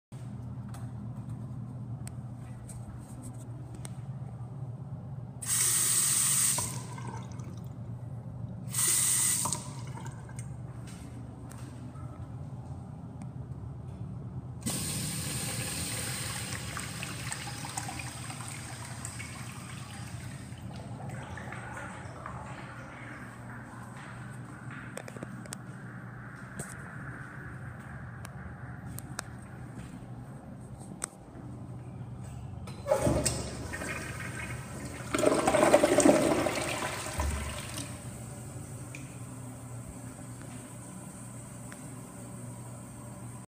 Newer Kohler fixtures
At a fountain place. Nice fixtures. Video filmed on December 27, 2016.